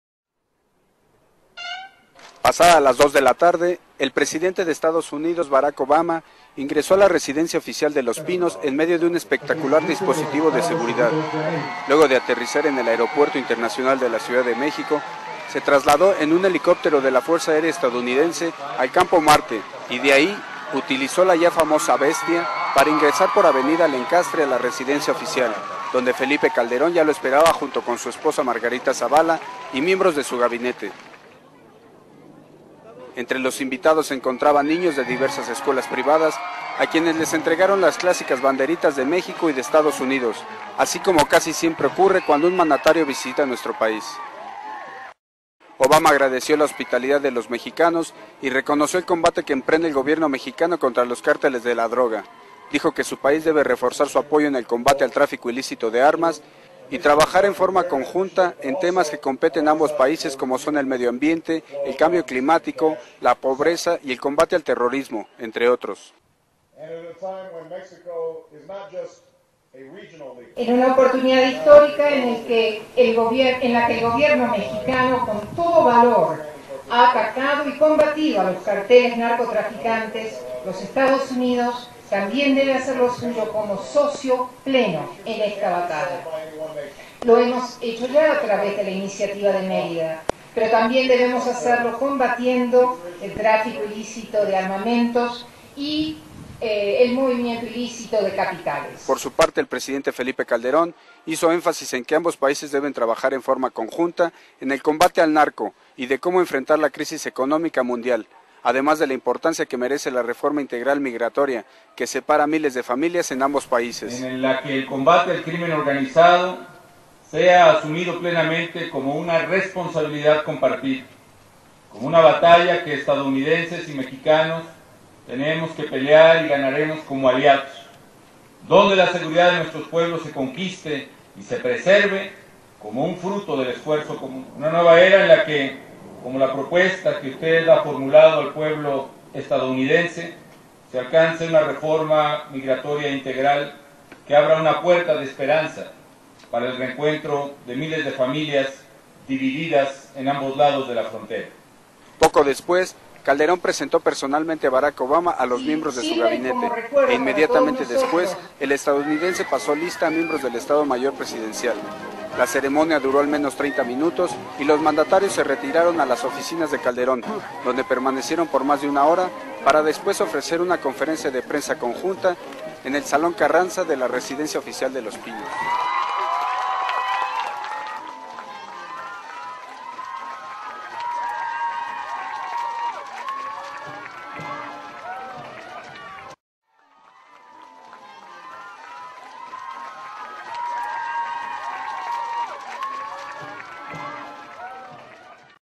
La primera vez de Barack Obama en México como presidente
El presidente de Estados Unidos se reunió con Felipe Calderón en la residencia oficial de Los Pinos en gira de trabajo